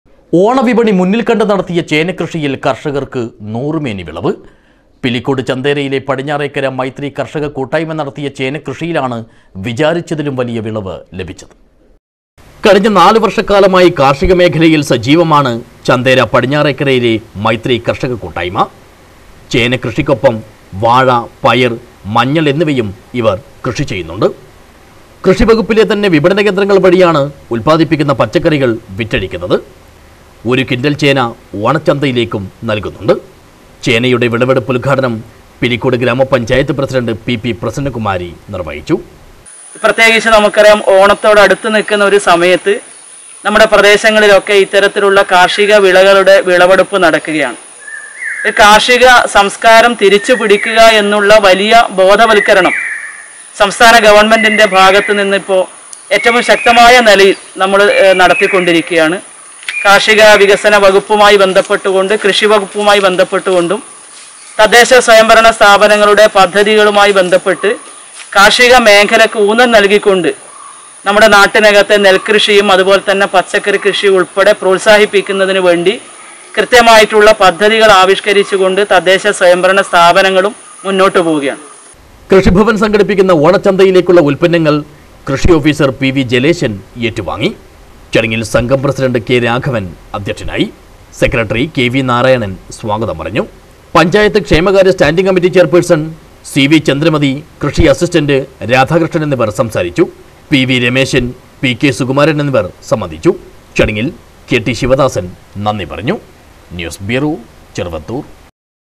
ഓണ വിപണി മുന്നില്‍ കണ്ട് നടത്തിയ ചേന കൃഷിയില്‍ കര്‍ഷകര്‍ക്ക് നൂറുമേനി വിളവ്
ഓണ വിപണി മുന്നില്‍ കണ്ട് നടത്തിയ ചേന കൃഷിയില്‍ കര്‍ഷകര്‍ക്ക് നൂറുമേനി വിളവ്. പിലിക്കോട് ചന്തേരയിലെ പടിഞ്ഞാറെക്കര മൈത്രി കര്‍ഷക കുട്ടായ്മ നടത്തിയ ചേന കൃഷിയിലാണ് വിചാരിച്ചതിലും വലിയ വിളവ് ലഭിച്ചത്.